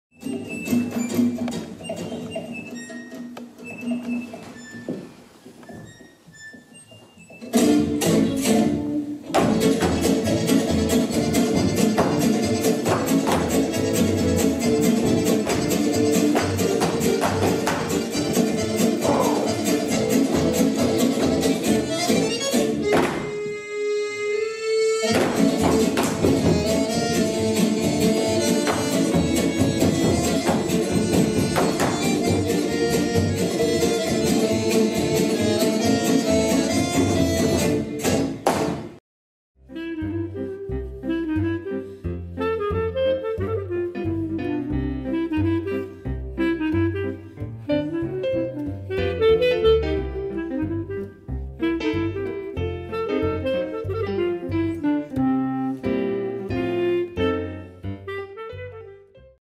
A TABLE ! / Extrait #3 : Bouillir
Chorégraphe : Amélie Boblin
Interprètes : Morlaye Camara, Margot Jouenne, Amélie Boblin
Création lumière : Nicolas Kergosien
Création sonore : Marion Vayssière
Graphisme, vidéo : Lise Nicolle
Décors : Nicolas Maze (Atelier du Chien qui Fume), Guillaume Cahue